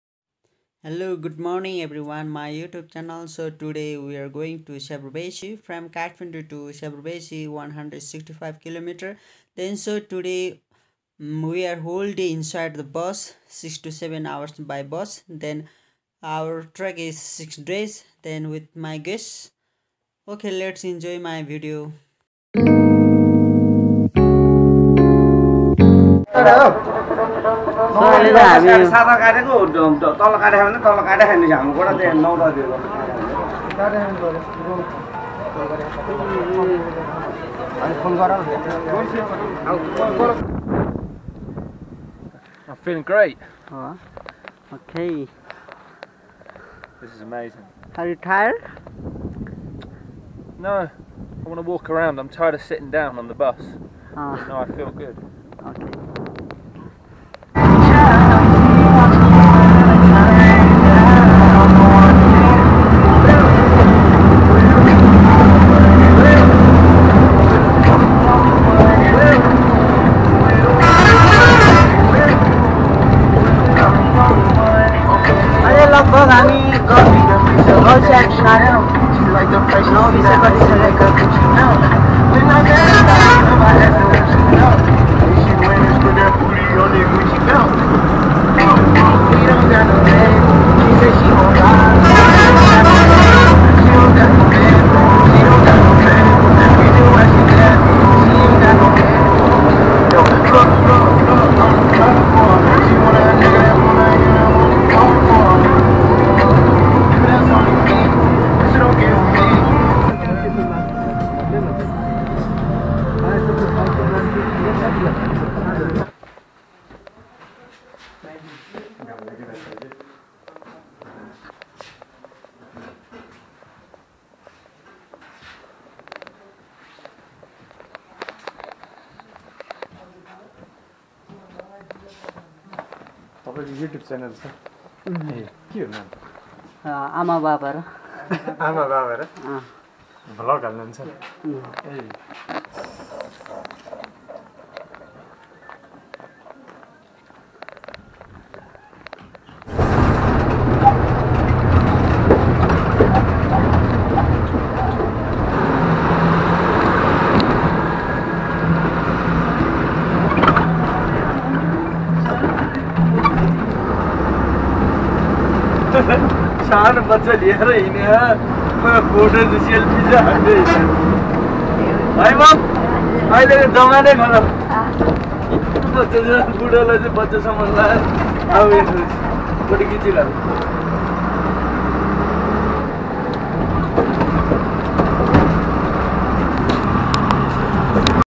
from Kathmandu to syapprubeshi traveling Langtang valley treks.Amababara.com
Langtong valley treks
 short trip of langtong valley treks

itinerary about langtong valley treks
first Day) KTM to syapprubeshi by bus 7 hrs.
second Day syapprubeshi to remche 5hrs
third day.   remche to langtong 6hrs 3430m.
fourth day. langtong to kenjingumpa 3hrs
and explore hiking kenjinri 4400m.
five days. kenjn gumpa to chyarkuri hiking 4985m and same way get back ghoda tabela. 12hrs.
six days ghodatabel to shyprubenshi 5hrs.
seven days shyprubenshi to KTM by bus 7 hrs..
                  Ending treks